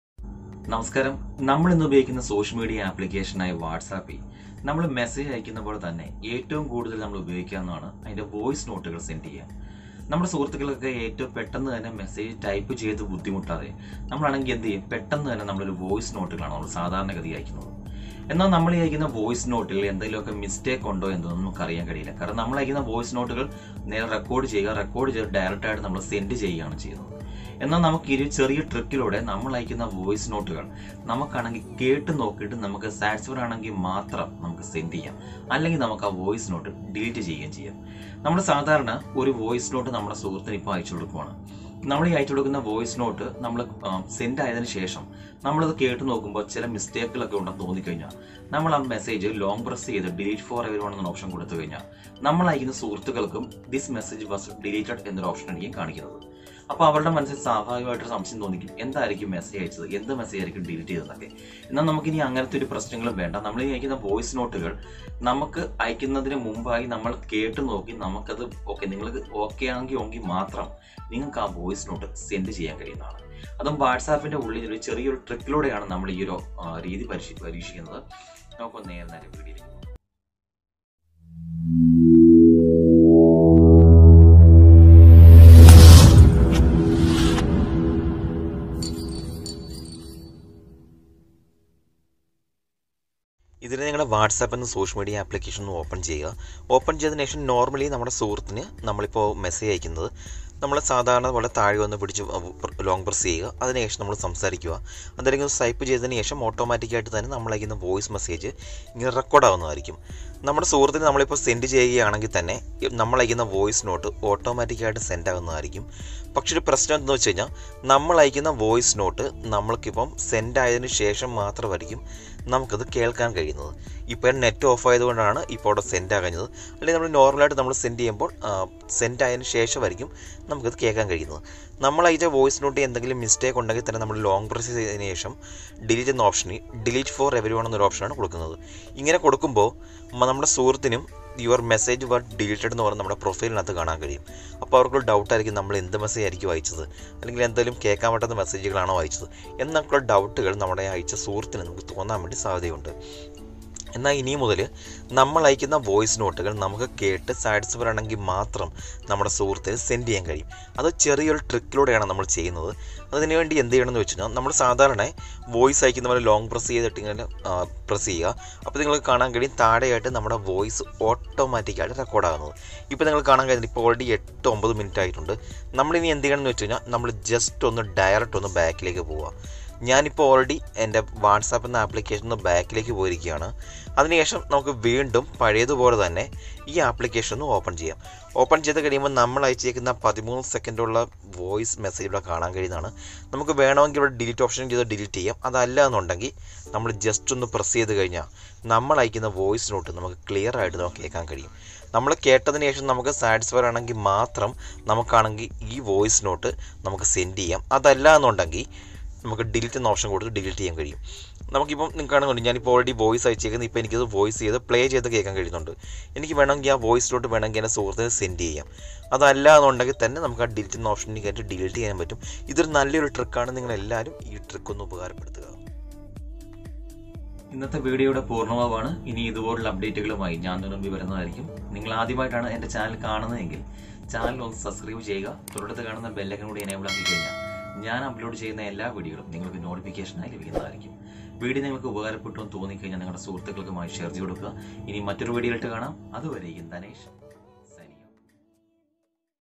WhatsApp Voice Send Before Listen👂| Tips and Tricks | DDR ALL in ONE
വാട്സ്അപ്പിൽ വോയിസ് അയക്കുന്നതിന് മുന്നേ കേട്ട് നോക്കി അയക്കാം